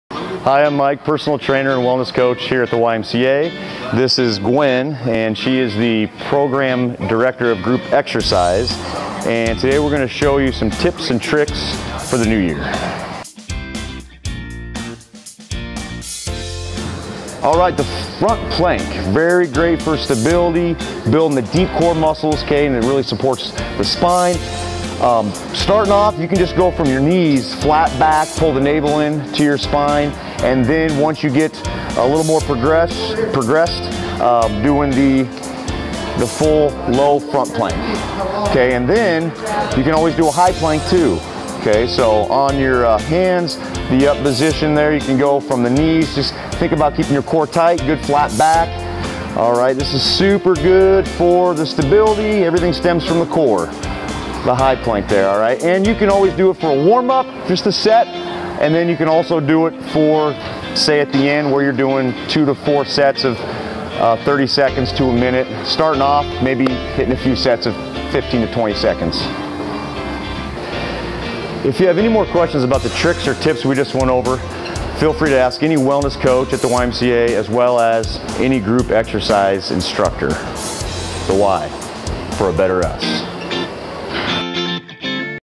12 Days of Fitness: How to Do a Front Plank
Mike and Gwyndolyn share the best way to do low and high front planks!